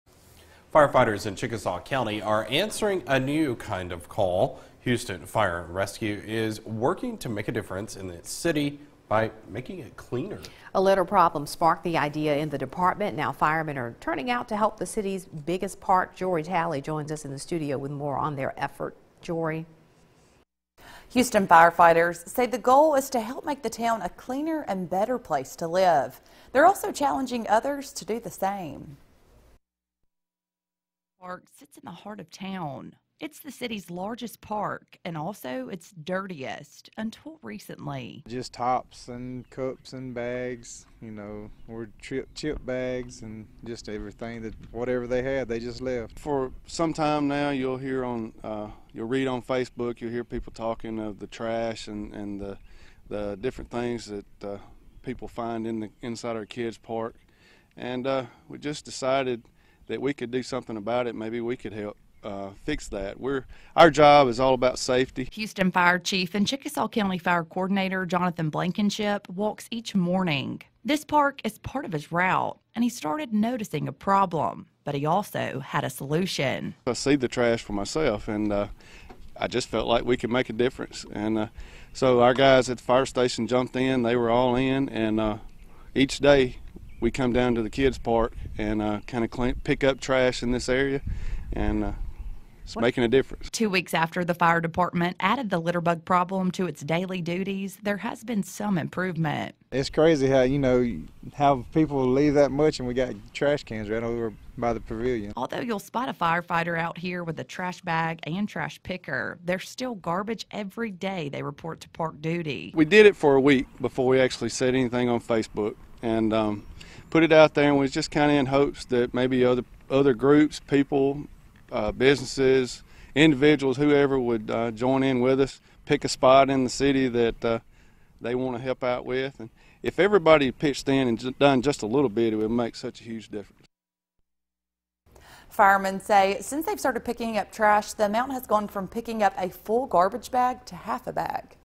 Community Litter
Firefighters in Chickasaw County are answering a new kind of call. Houston Fire Rescue is working to make a difference in its city by making it cleaner.